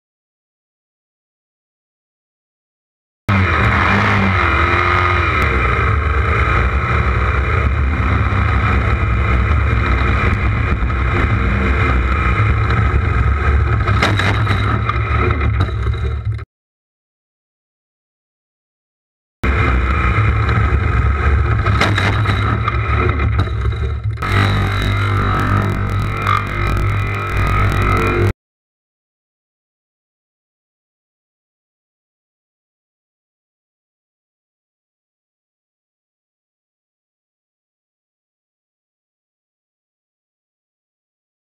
Blues Crash
Broke my collar bone when I low-sided my Yamaha Super Tenere in the Blue Mountains, WA.